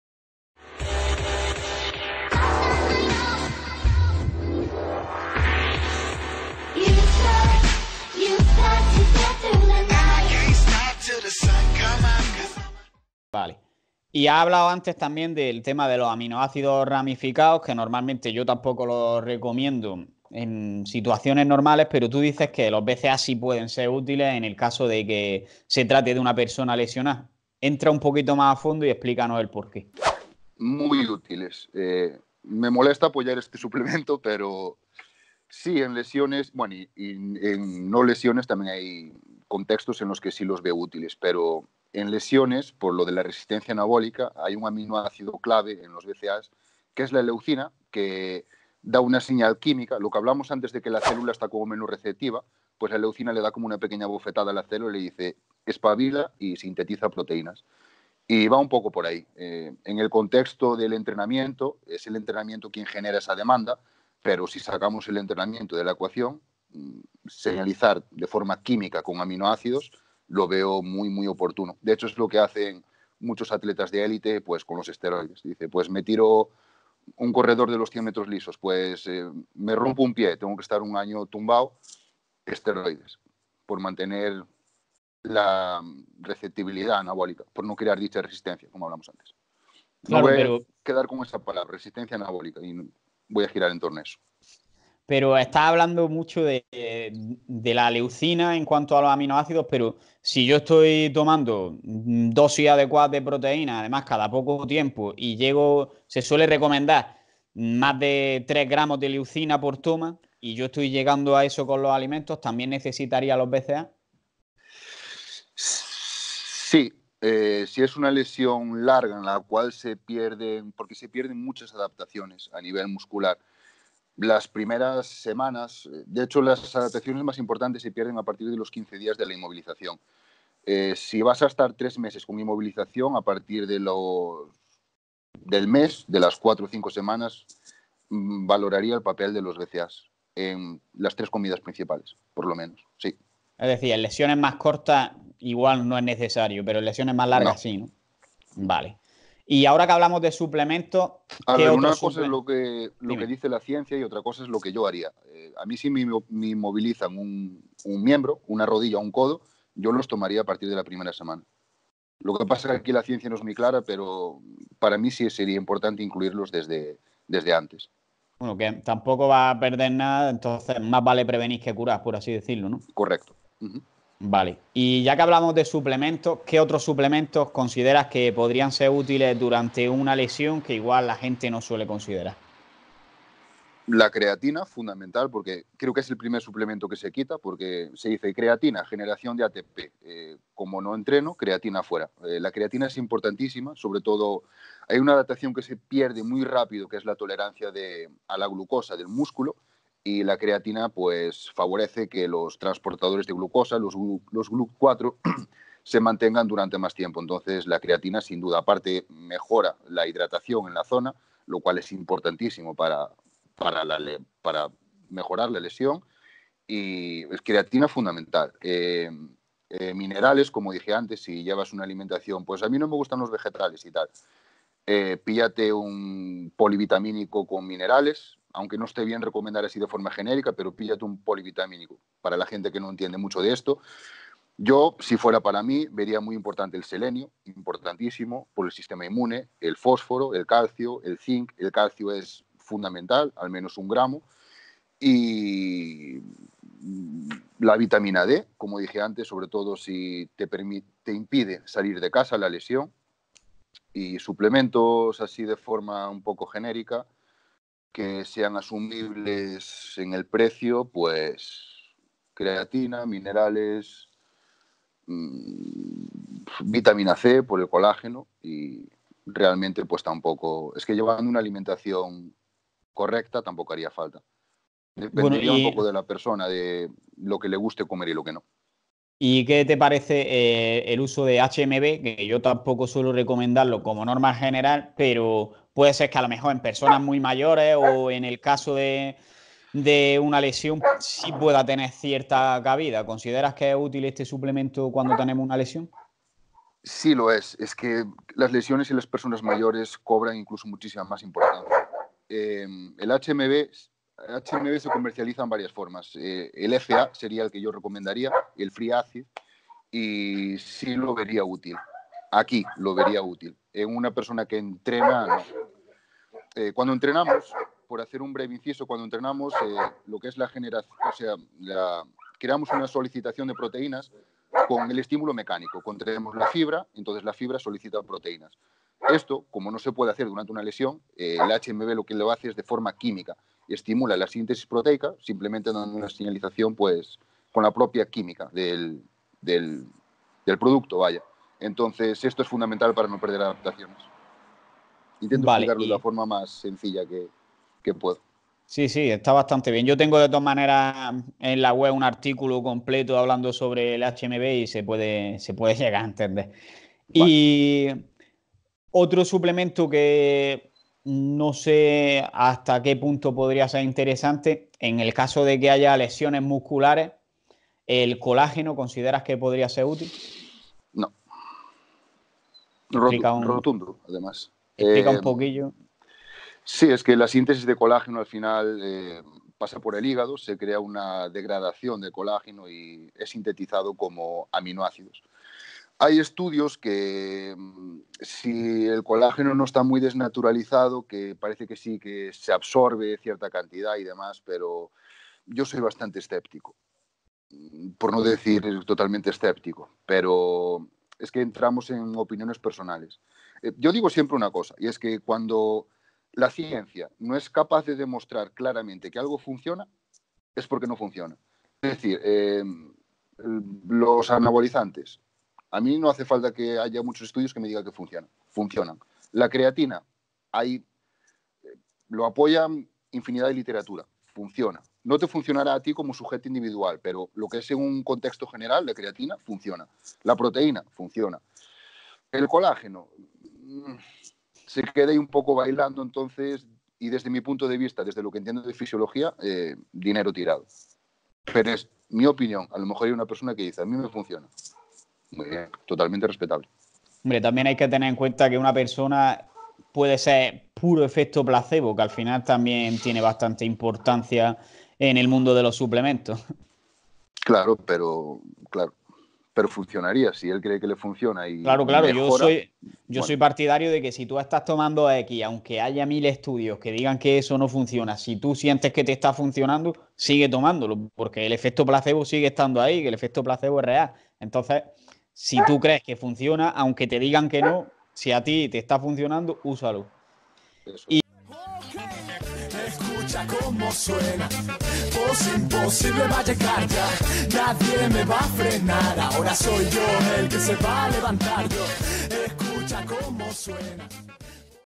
⚡ SUPLEMENTOS para recuperarse de una LESIÓN ⚡ Con CULTURA DE GYM
PODCAST COMPLETO 🎧🎧🎧

SOBRE MARCOS

CONTENIDO RECOMENDADO  📚

El libro Negro del Entrenamiento - Christian Thibaudeau
Mi Dieta Cojea - Aitor Sánchez